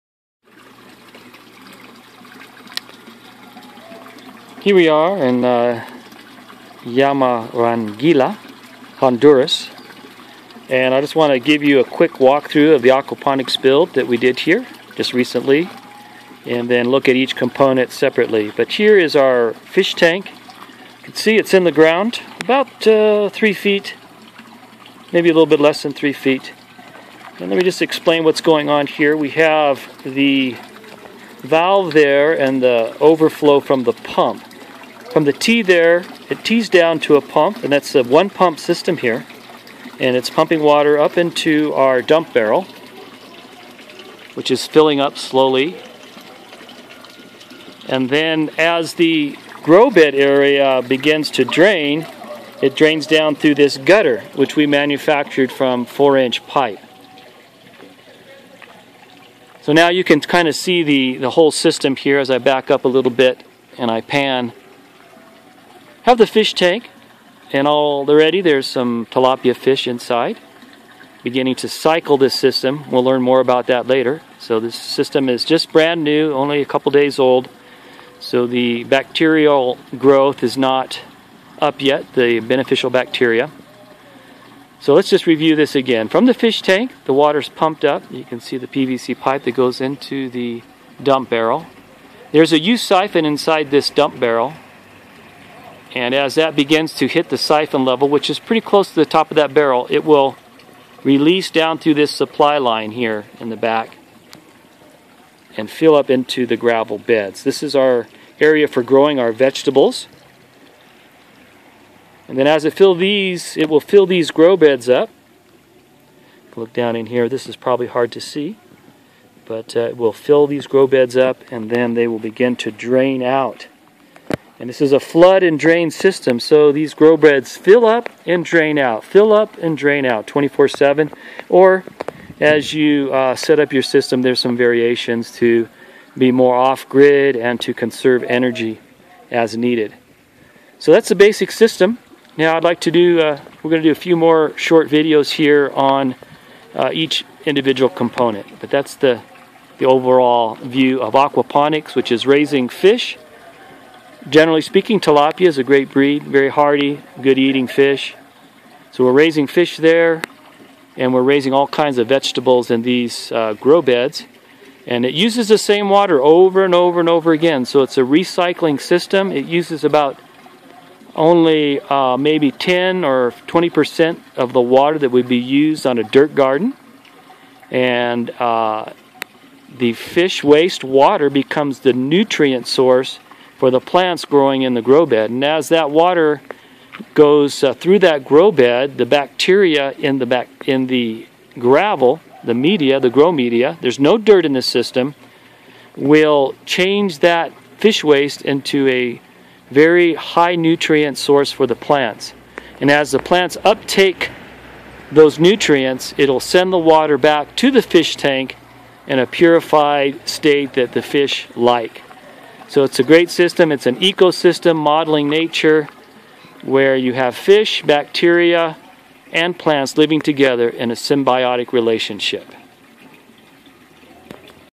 Components of the Aquaponics System
Here is a description of the basic components of the system.  It is called the 5 minute system because of a wildly popular time lapse video of the original build.  Personal thanks goes to Travis Hughey the originator of Barrel-Ponics, and further training from Dr. Bonnie Hanszen.